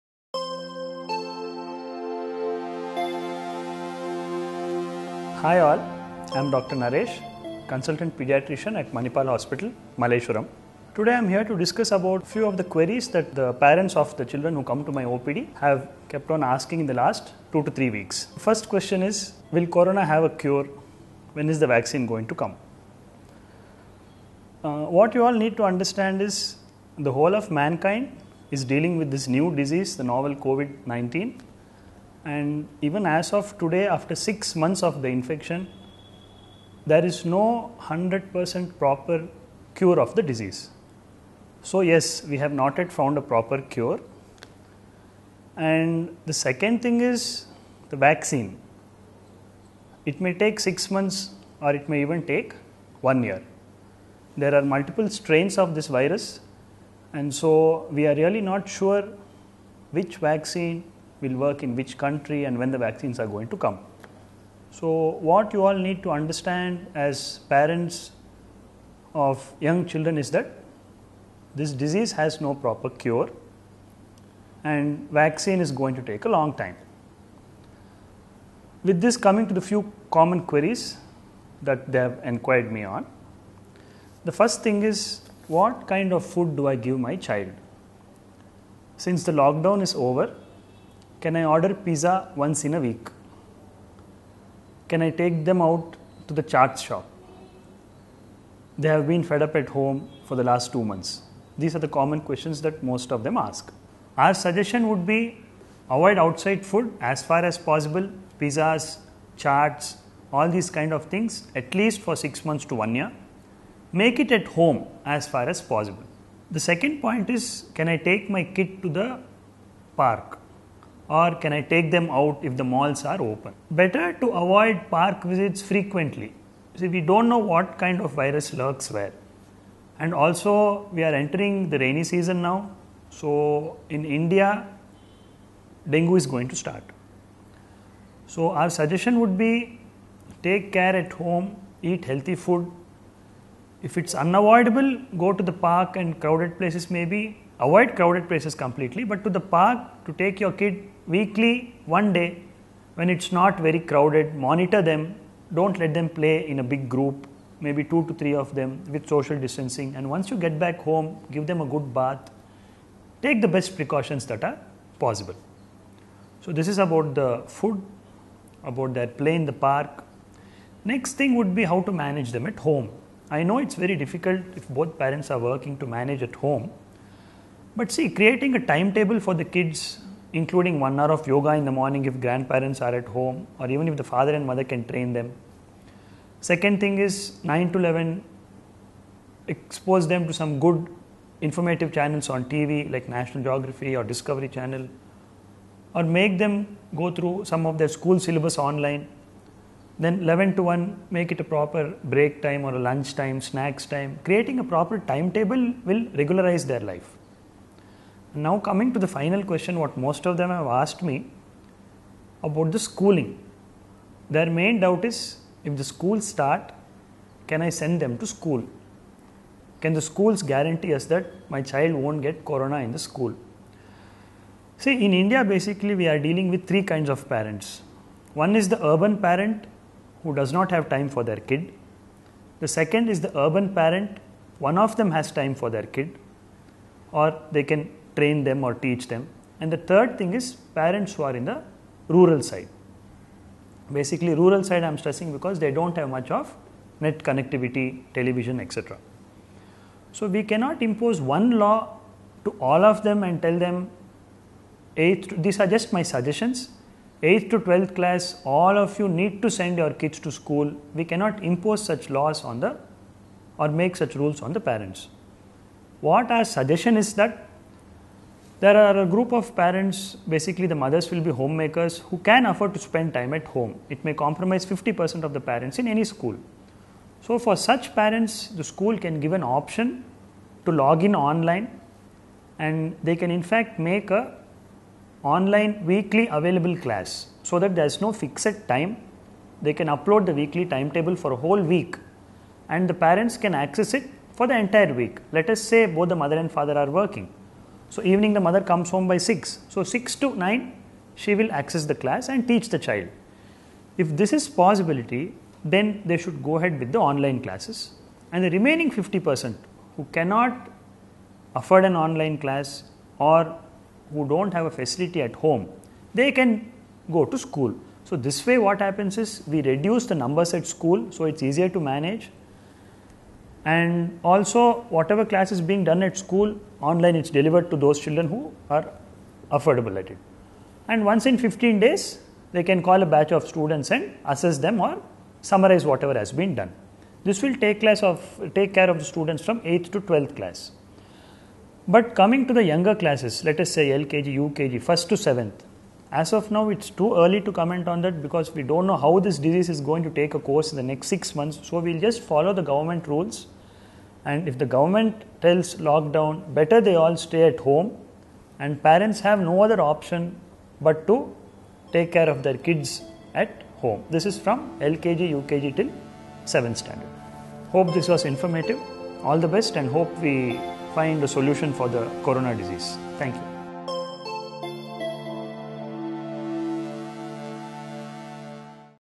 Dr. Naresh Pagadala | Importance of home-cooked food | Manipal Hospitals India
Avoid stepping out unless it is absolutely necessary. Try to replace store-bought food with home-cooked food for everyone, especially children, says Dr. Naresh Pagadala, Consultant- Paediatrics, Manipal Hospitals, Malleshwaram. Watch the video to know more!